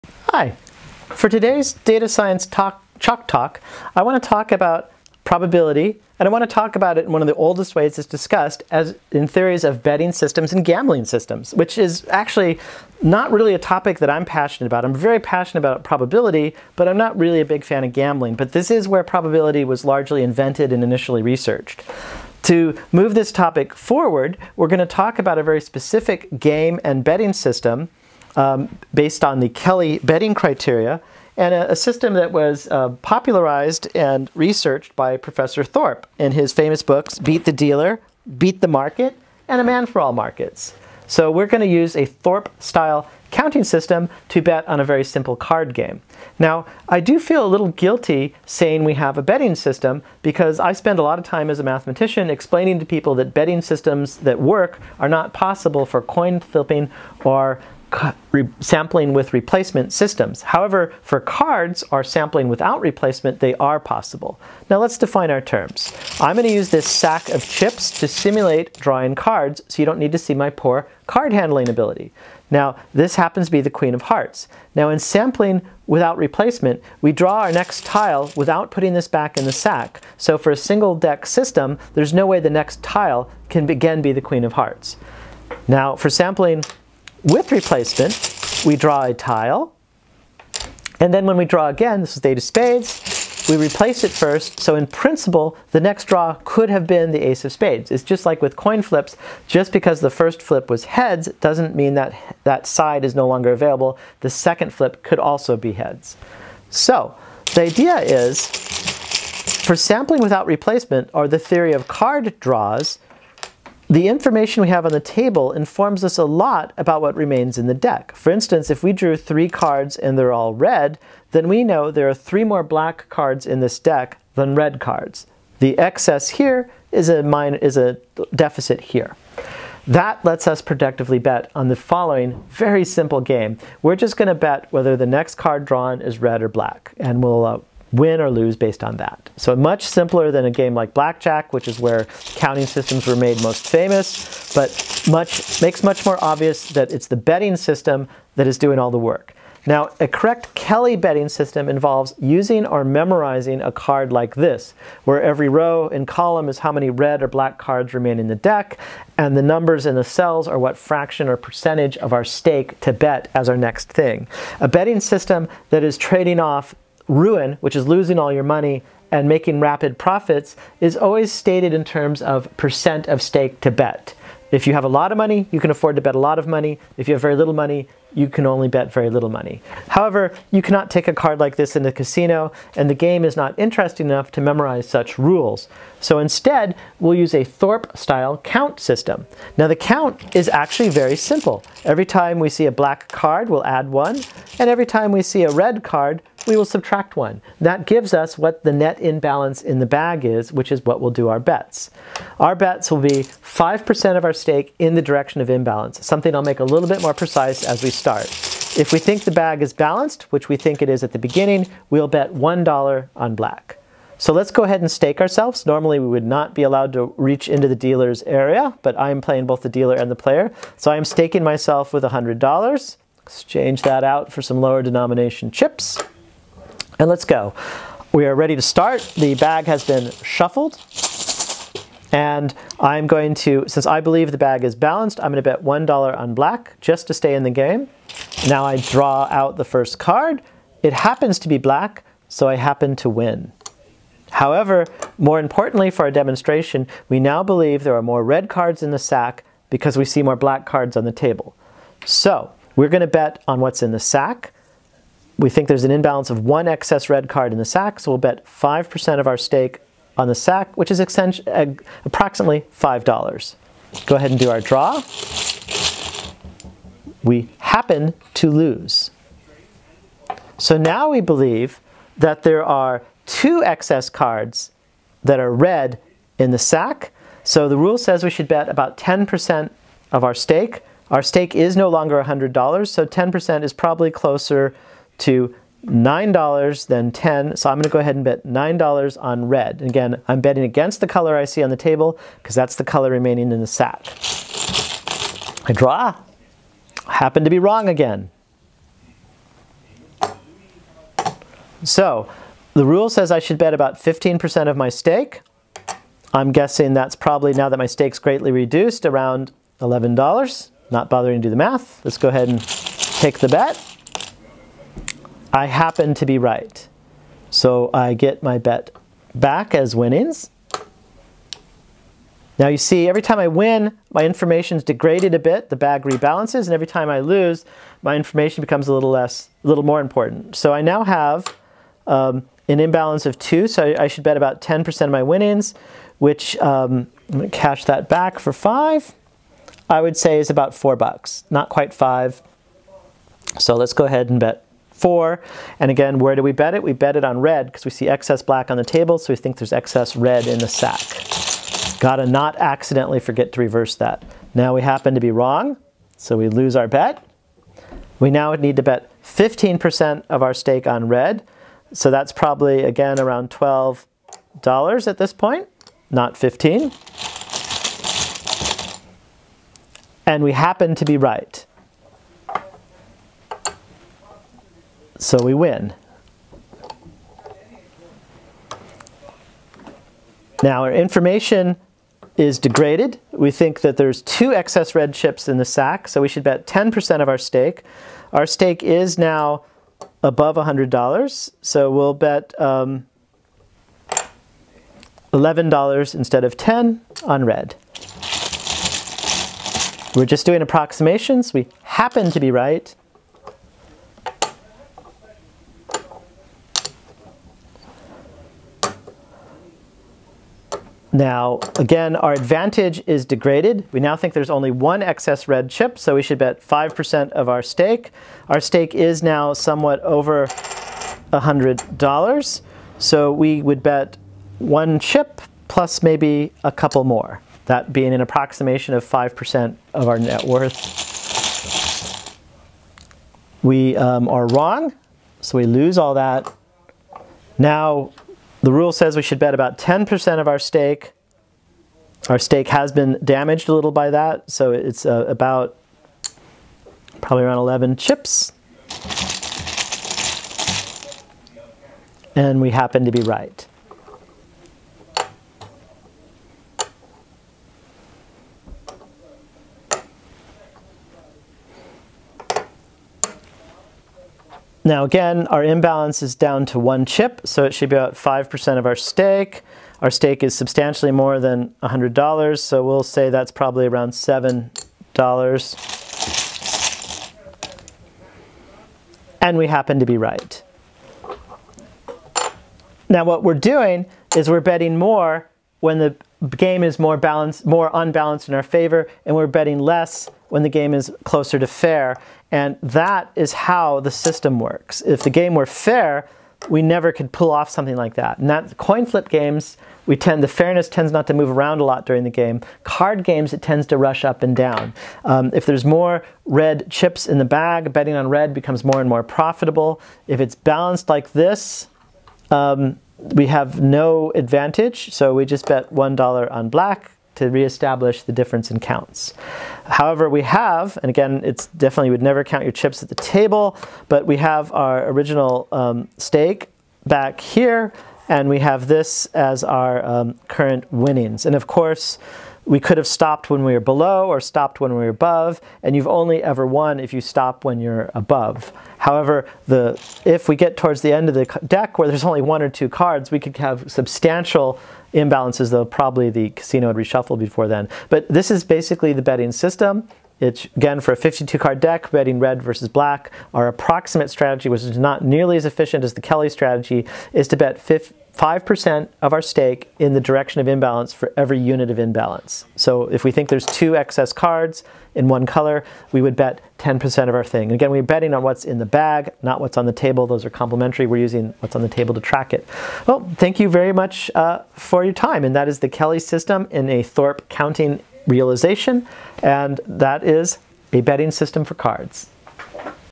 Kelly Thorp Betting
An example of using a Kelly / Thorp betting system. A write-up of the derivation of the method can be found here:

I also have a note on why these methods don't apply to fair coin games (or sampling with replacement):